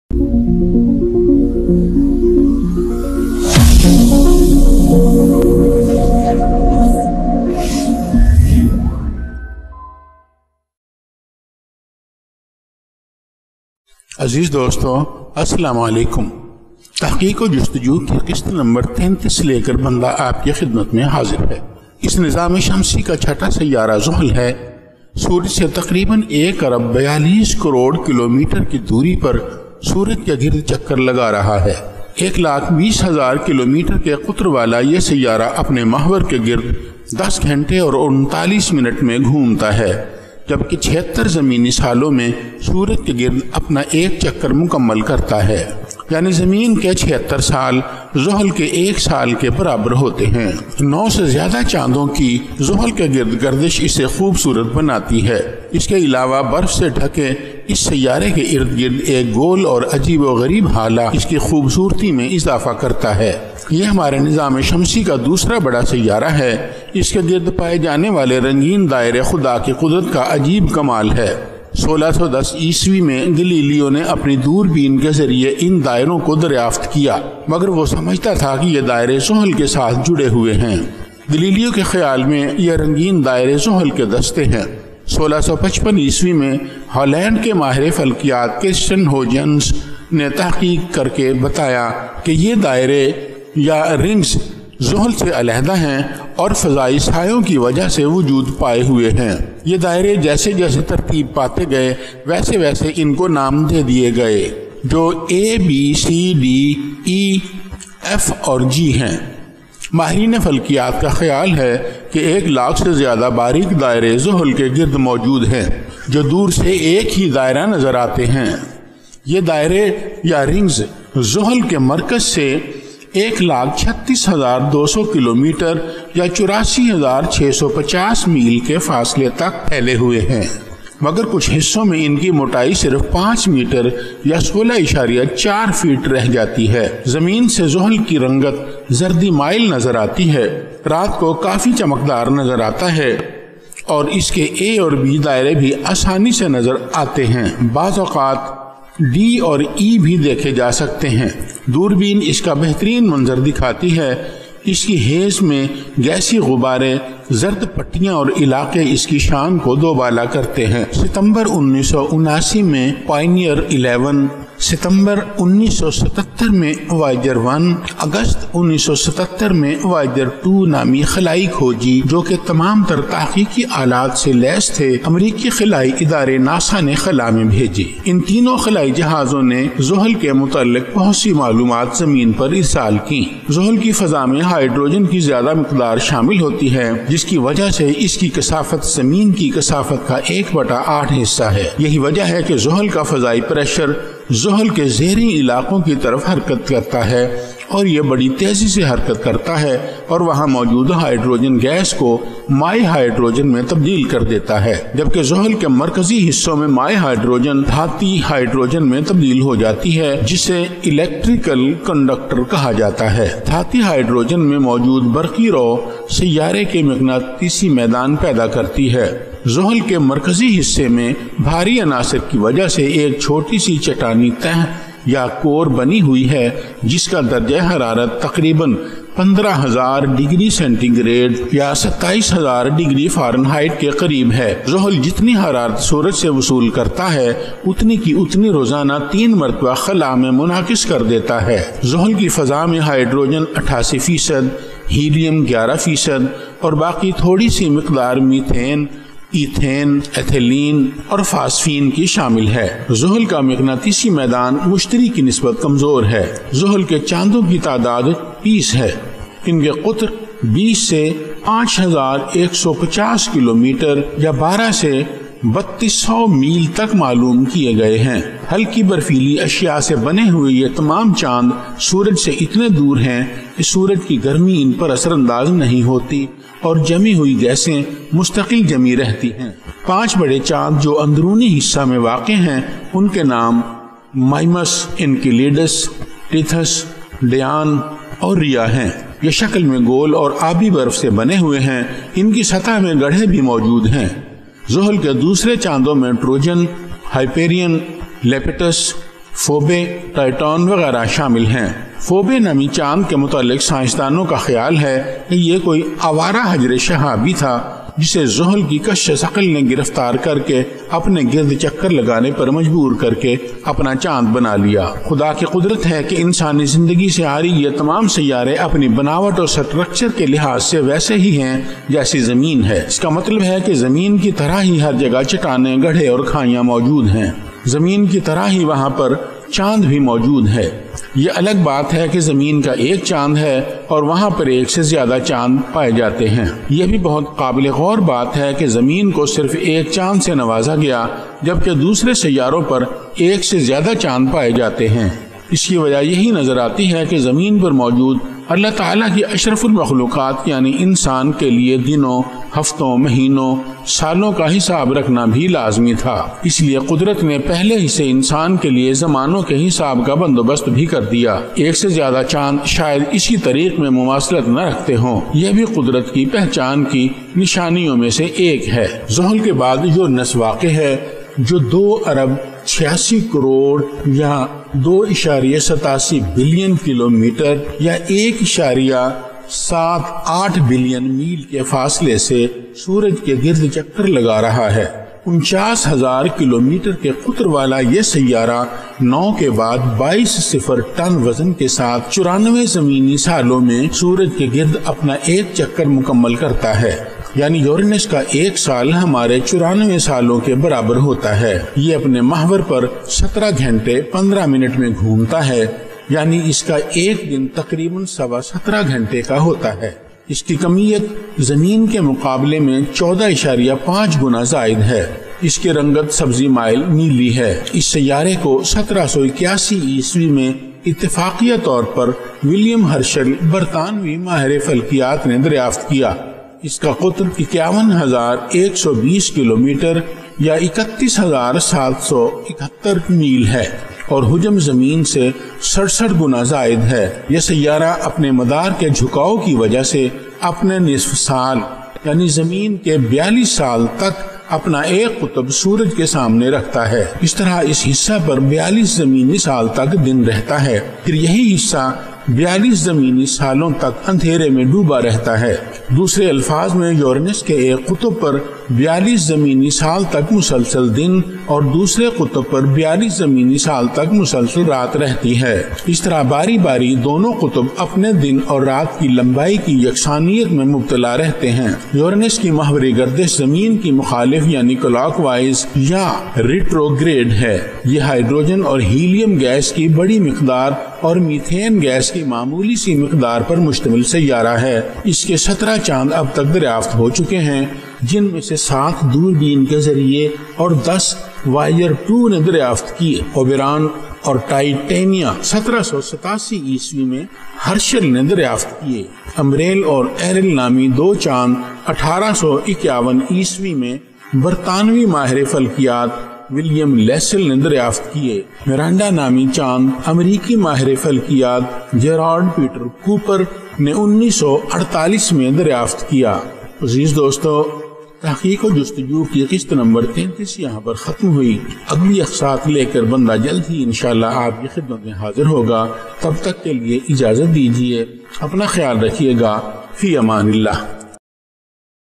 Tehqiq o Justju 33
It is description of two planets of our Solar system.
Saturn and Uranus are two planets occurred in the outermost part of our Solar system.